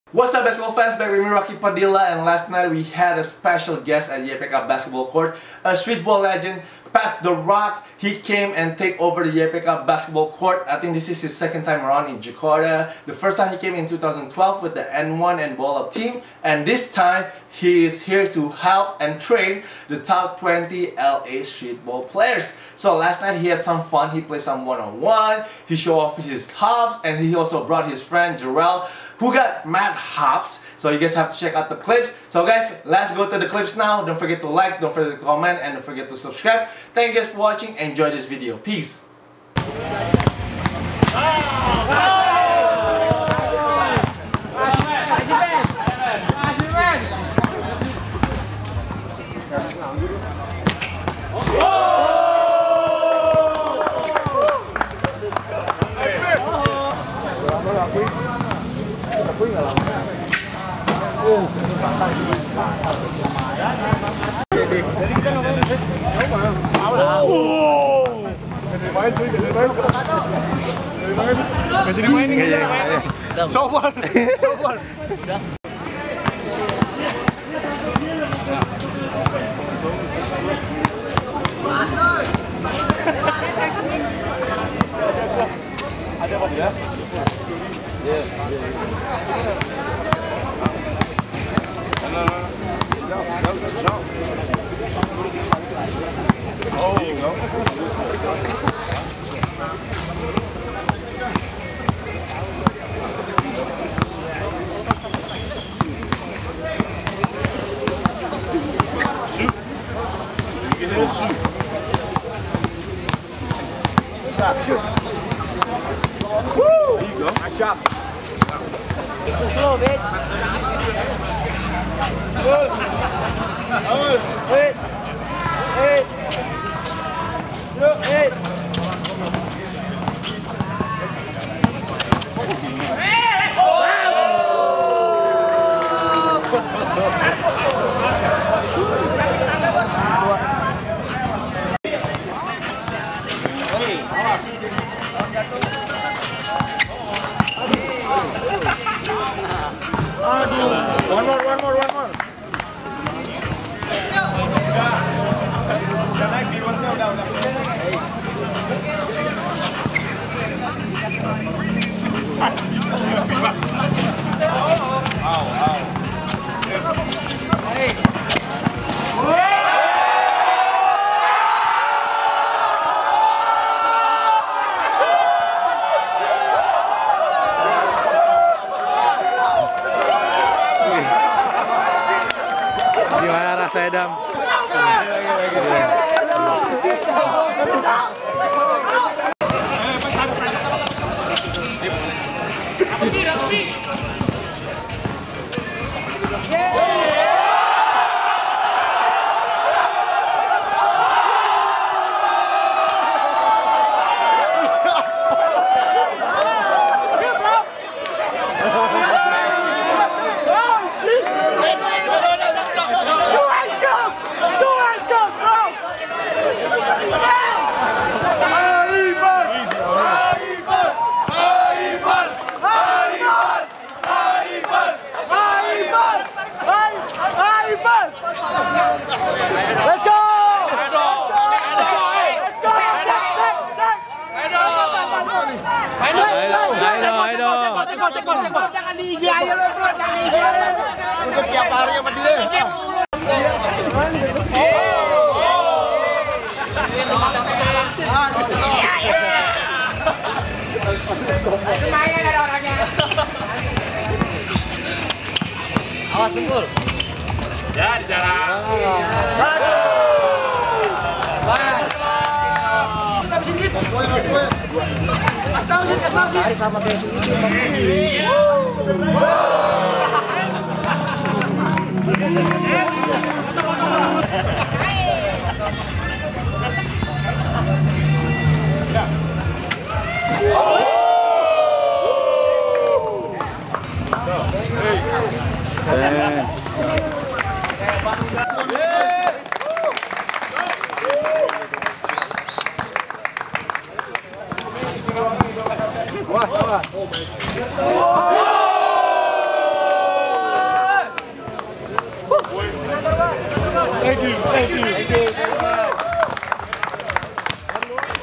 Pat The Roc Takes Over the YPK Basketball Court & Breaks Ankles.
Pat The Roc takes over the YPK Basketball Court and played some one-on-one and also shows off his hops.